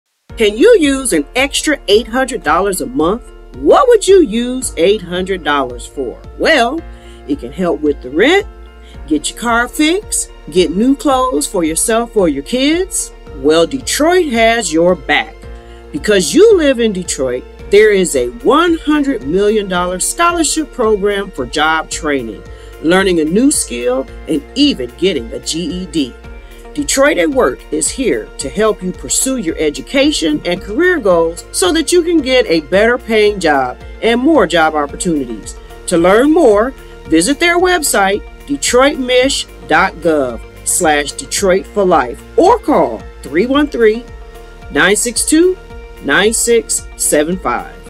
Detroit For Life: Offering Limitless Opportunities
Detroit at Work is here to help you pursue your education and career goals so that you can get a better-paying job and more job opportunities.

If you want to be a guest on the show and have a story to tell...

Please call, write or email the show!!